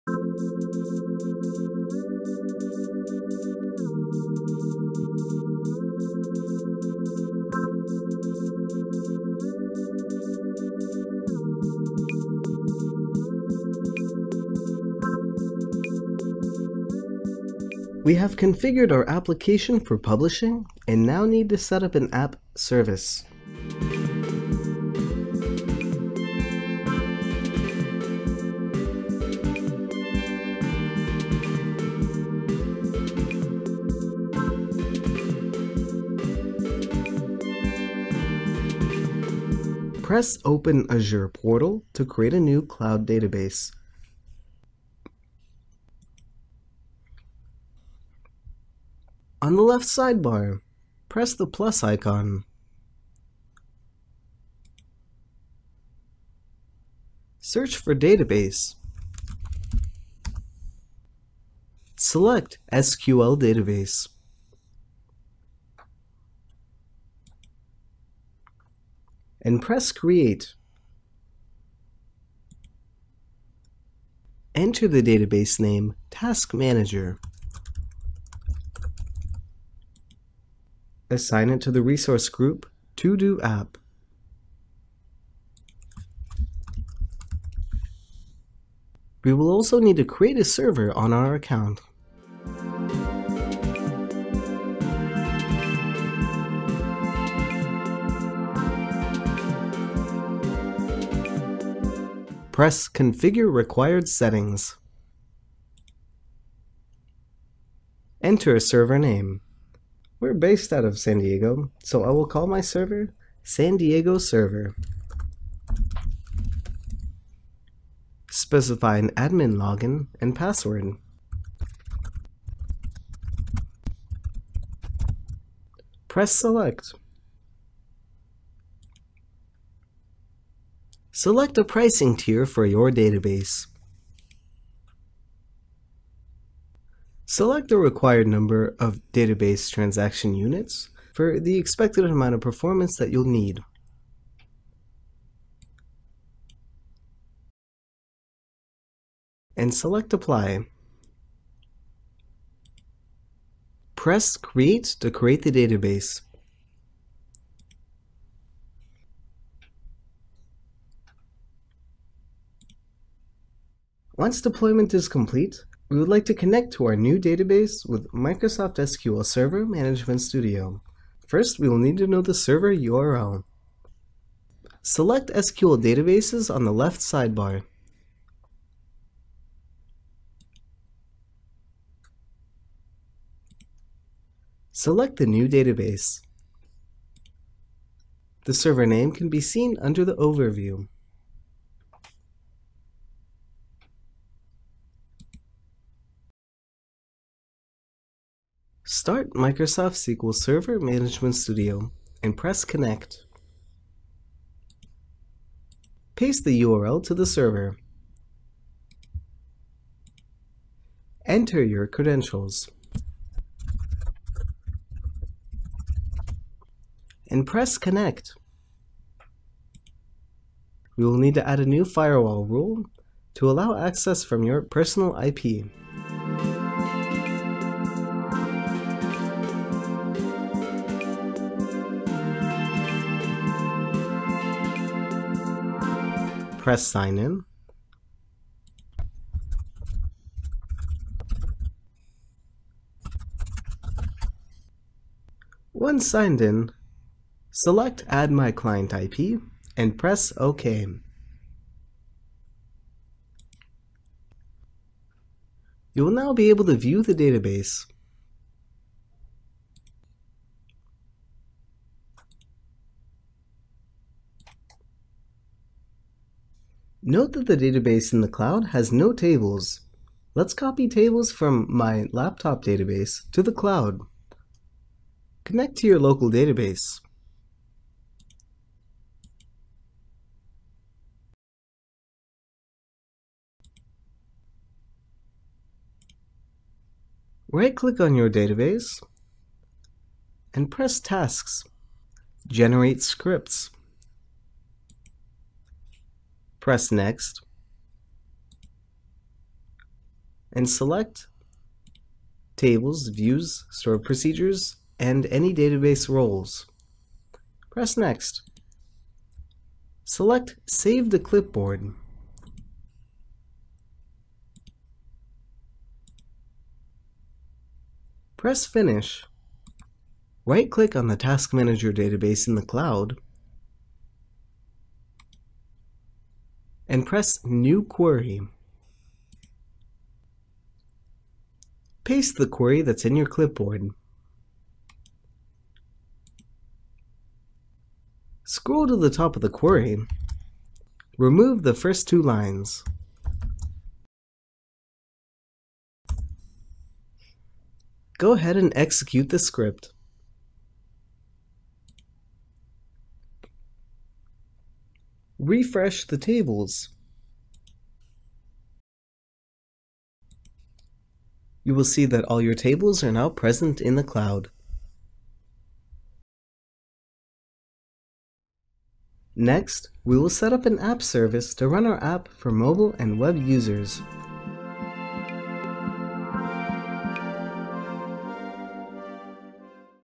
ToDo App Episode 13: Cloud Database
An application deployed to Microsoft Azure is typically configured as Web App. The deployment infrastructure of Web App includes an Internet-facing App Service and Database Server. In this episode we will set up a database server and transfer development database schema to the cloud database.